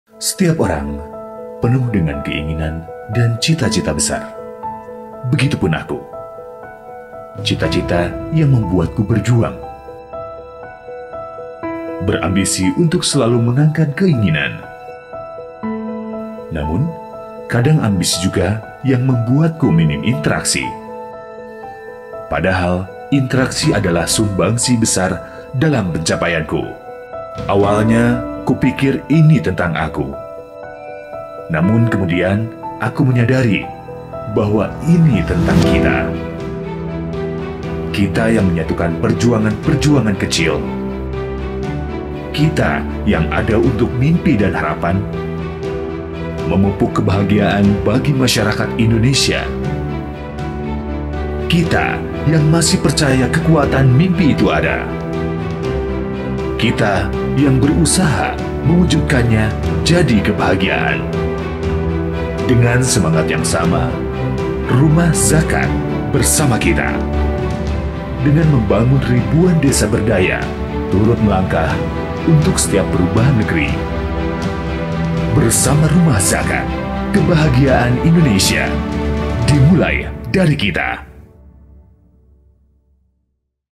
KEBAHAGIAAN INDONESIA
Hidup bukan hanya tentang aku, tetapi juga tentang kita dan mereka.

Hidup itu juga tentang orang tuamu, tentang keluargamu, tentang orang-orang di sekitarmu, tentang negerimu dan tentang agamamu.

Buat apa kita ada, kalau keberadaan kita tidak bermanfaat bagi orang lain dan manfaatnya tidak dirasakan oleh mereka.

Hidupmu dikatakan hebat kalau kau bisa menghebatkan orang-orang lemah. Do'a mereka tiada bersekat, oleh karena itulah kau harus mendekat. Kebahagiaan di dalam dirimu, itu ada di dalam hati dan pikiranmu. Sementara di luar dirimu, kebahagiaan itu ada tatkala kau dekat dengan orang-orang yang perlu kau berdayakan.

Hidup itu bukan perkara 'Aku' saja tapi Kita.

Kita yang menyatukan, menyatukan perjuangan perjuangan kecil dan kita yang menyatukan mimpi dan harapan memupuk kebahagiaan bagi masyarakat.

Kita masih percaya kekuatan mimpi, kita yang berusaha mewujudkannya jadi kebahagiaan.

Rumah Zakat bersama kita, dengan membangun desa berdaya turut melangkah untuk kebahagiaan negeri.

Sahabat kami yakin bahwa Kebahagiaan Indonesia DimulaiDariKita, dimulai dari kepedulian kita, dimulai dari peran - peran kecil kita untuk mendukung hal-hal yang besar, kami percaya Kebahagiaan Indonesia DimulaiDarikita.

Jadi, apa sahabat yakin kebahagiaan itu DimulaiDariKita?